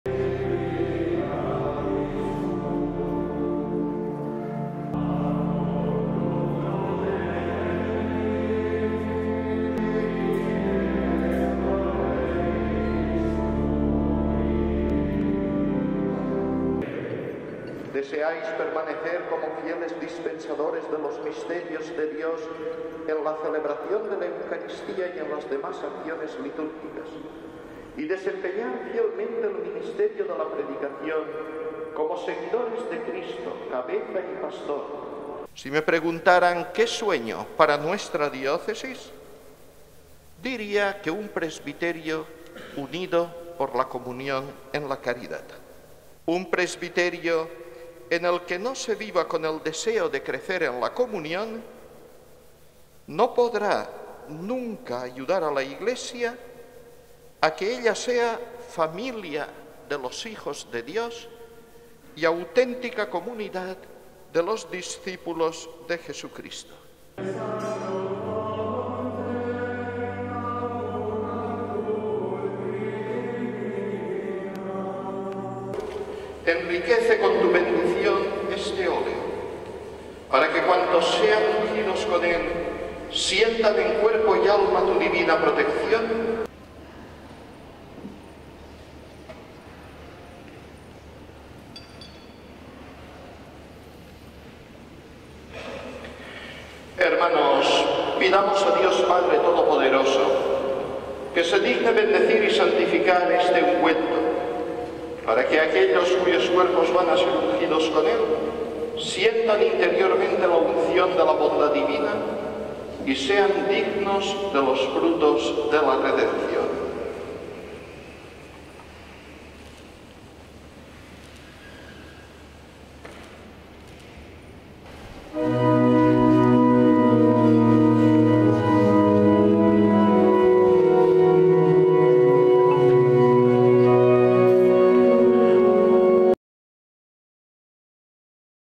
El arzobispo de Valencia, Enrique Benavent, preside la Misa Crismal en la Catedral de Valencia
El arzobispo de Valencia, Enrique Benavent, ha destacado que “si me preguntaran qué sueño para nuestra Diócesis diría un presbiterio unido por la comunión en la caridad” y para conseguirlo es “fundamental reforzar los vínculos de comunión entre el Obispo y los sacerdotes”, algo que sucede “especialmente” en la Misa Crismal que ha presidido esta mañana en la Catedral.